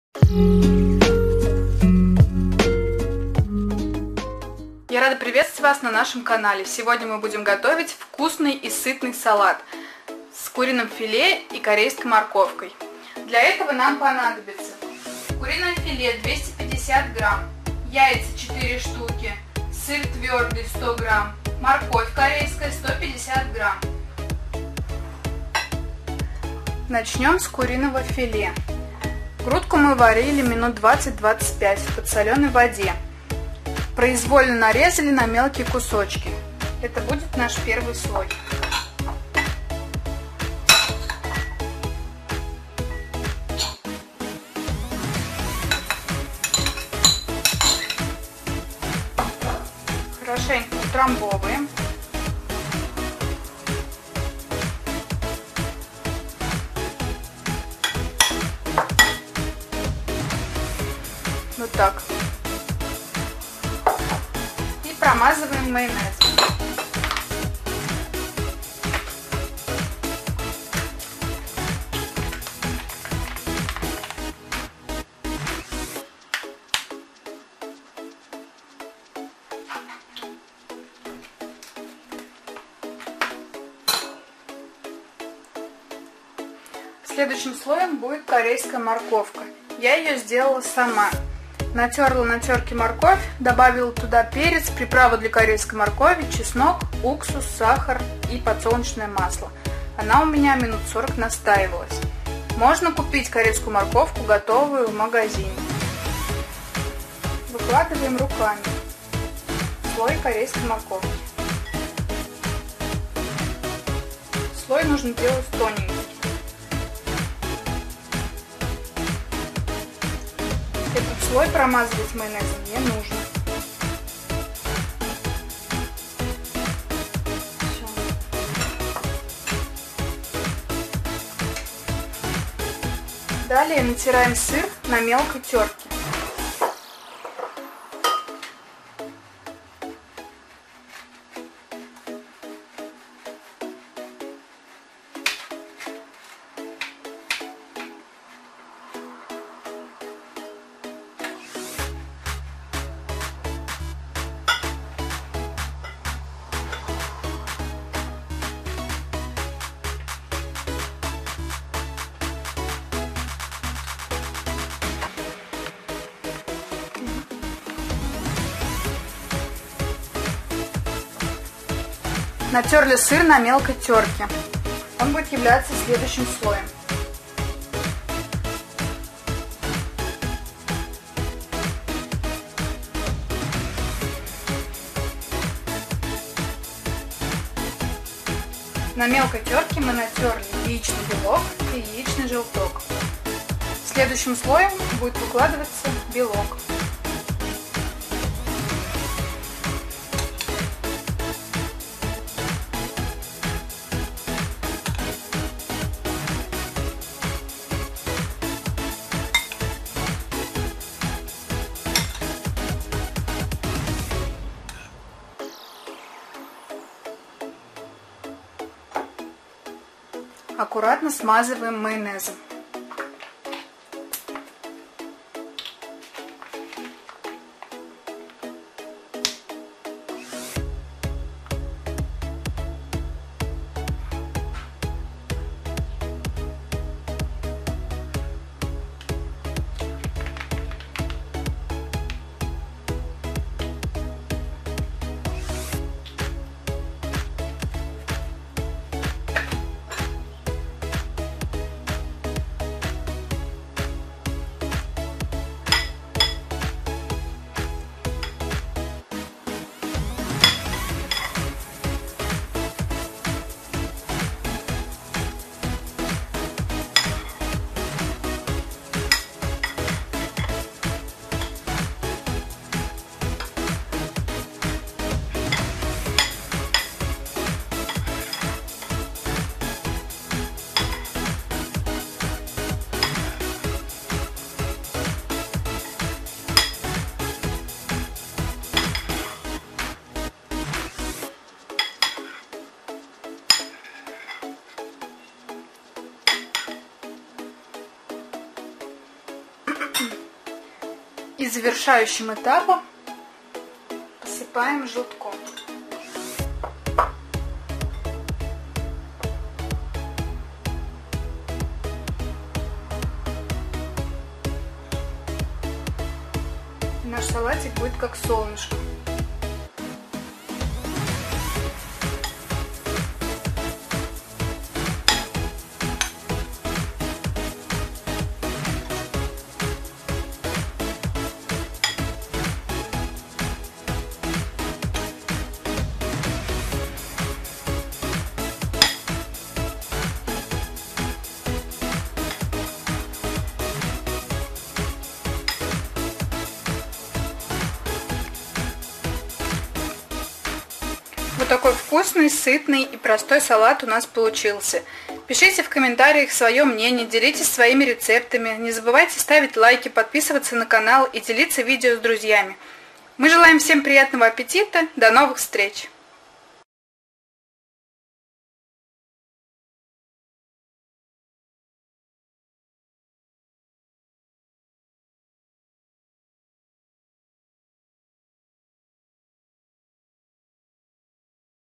♥♥ Салат "НЕЖНОСТЬ" Съедают Первым На Столе! Очень Вкусно!♥♥
♥♥Продукты
Филе куриное - 250 г
Яйца - 4 шт. + яйца для украшения (по желанию)
Приправа для корейской моркови - 1 ст. ложка
Зелень свежая для украшения - по вкусу